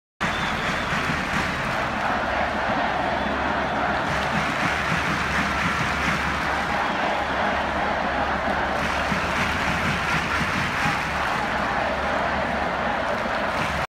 21 oct, vista aérea manifestación de Barcelona
Vista aérea de la manifestación el 21 de octubre en Barcelona en la que se pide la exarceración de Jordi Sánchez y Jordi Cuixart, presidentes de ANC i Ómnium Cultural.

Vídeo difundido en twitter por @peigis